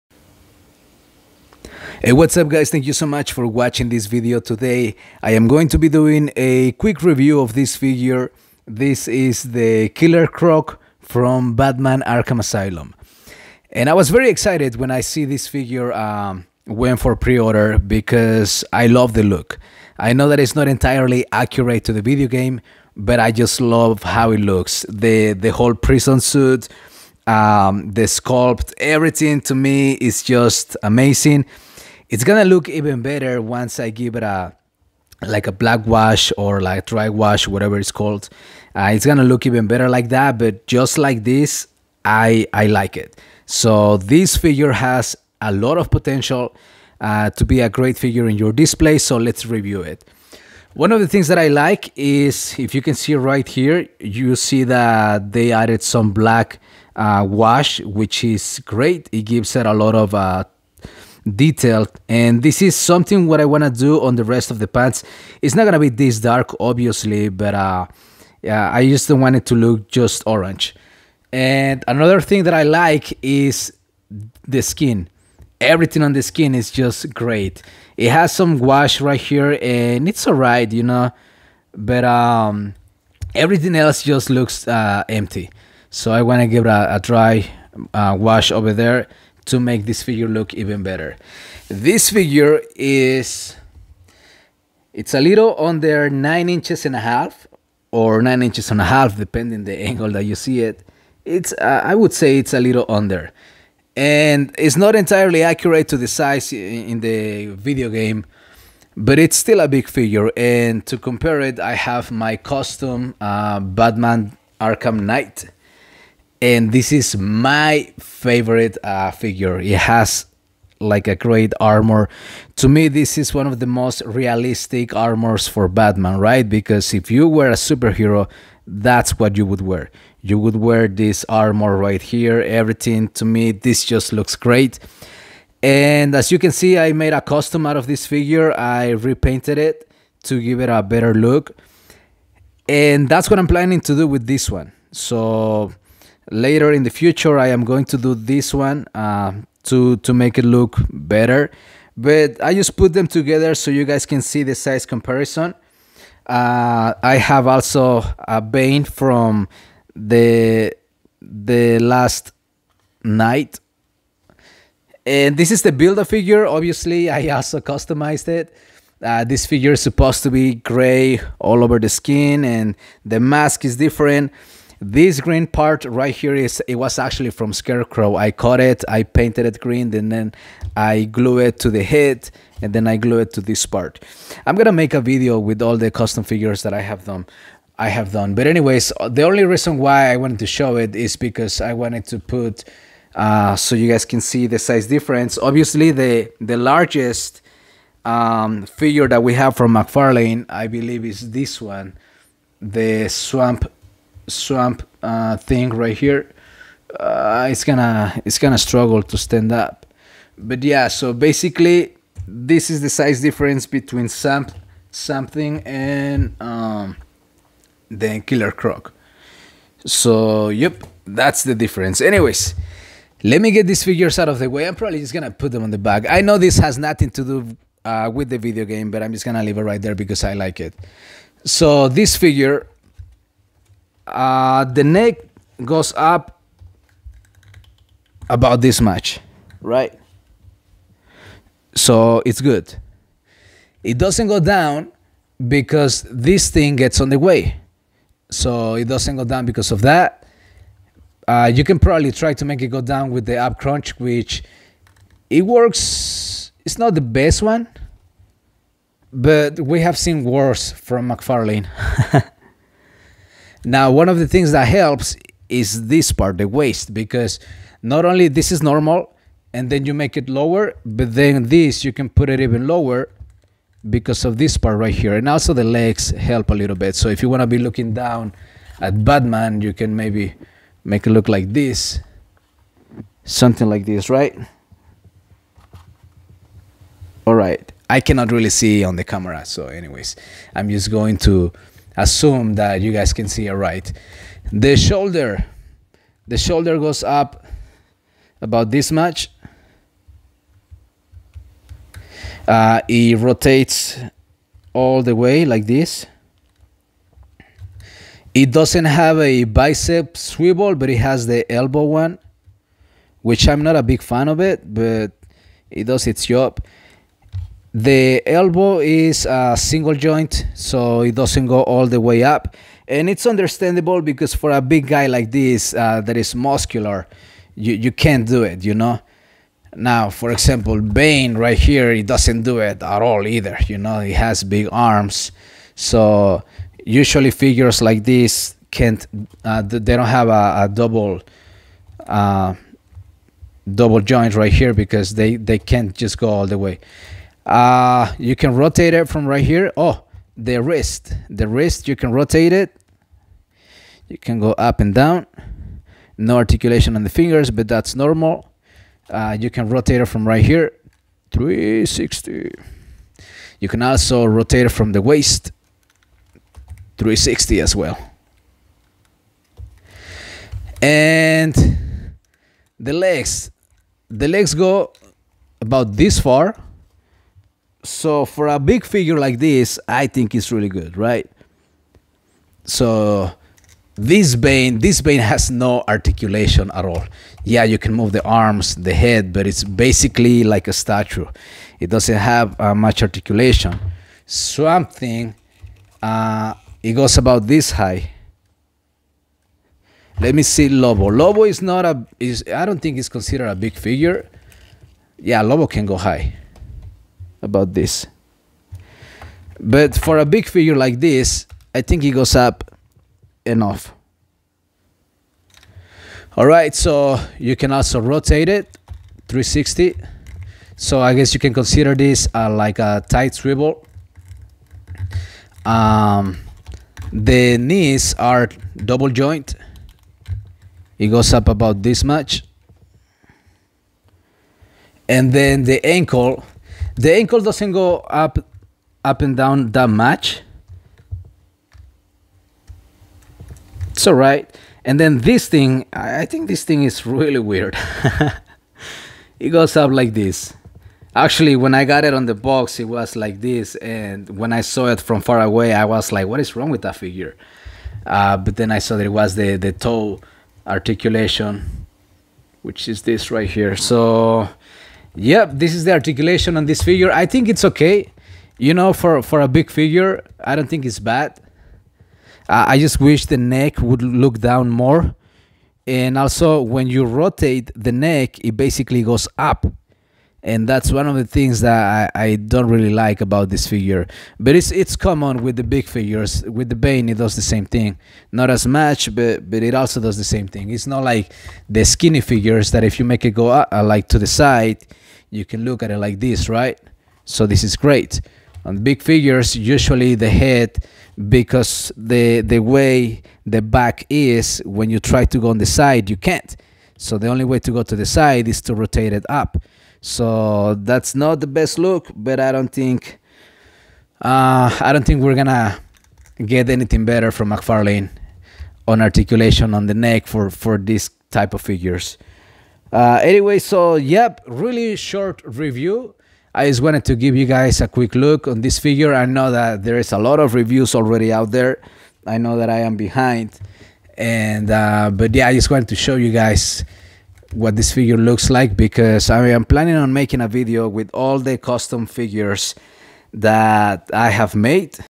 Mcfarlane DC Multiverse Killer croc , Batman Arkham Asylum Action Figure Review
hey guys this is a quick review of the Killer croc figure from Mcfarlane DC Multiverse Arkham Asylum